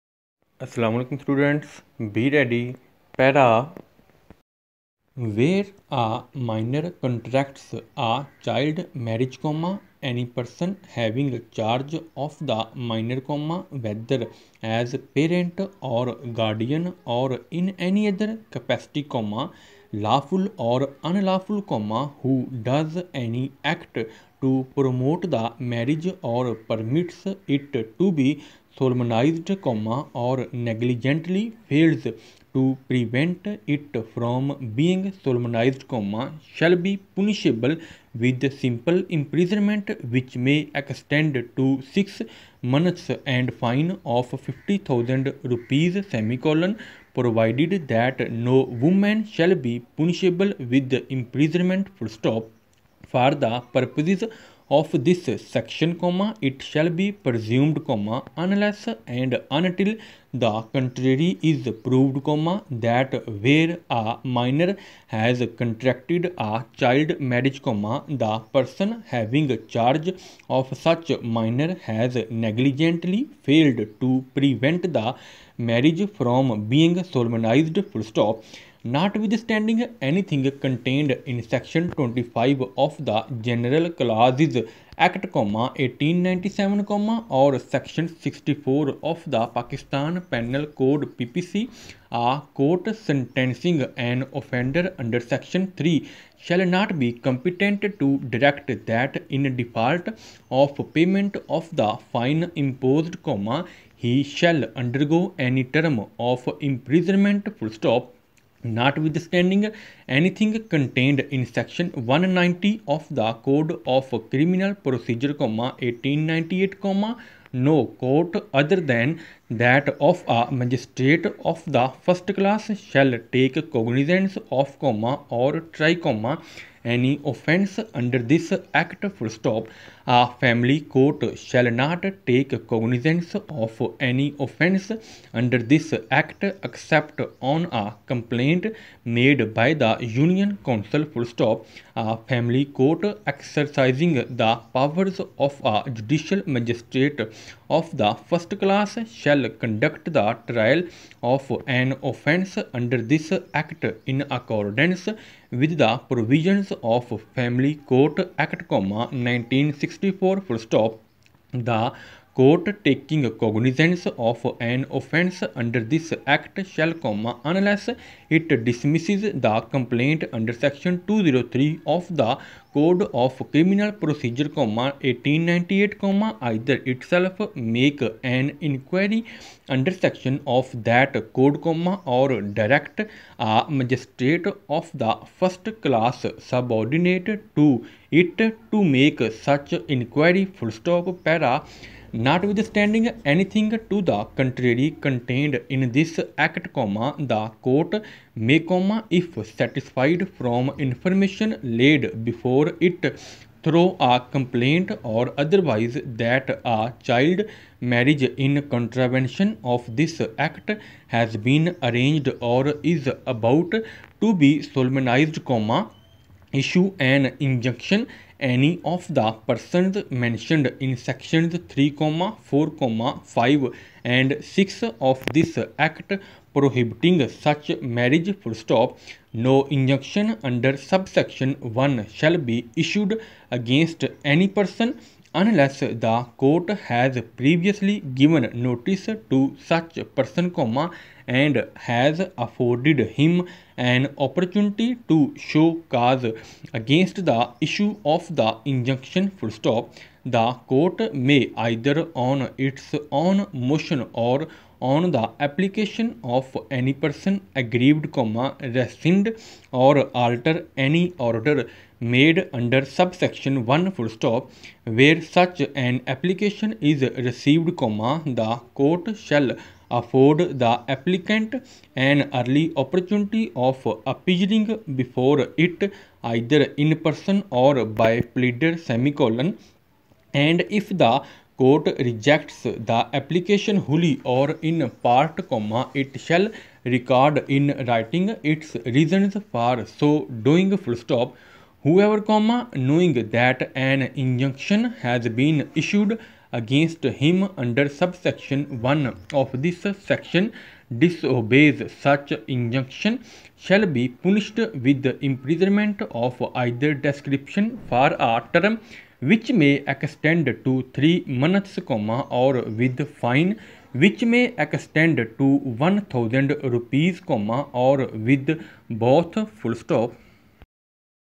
Ajmair Official Para No.14 @sohailshouket | @100 WPM | Ajmair Shorthand Dictation
Dictation of Ajmair Official Passage No.14 @sohailshouket | @100 WPM | Shorthand Speed of Ajmair |
If you want to get complete Book of Ajmair Shorthand Guide, click the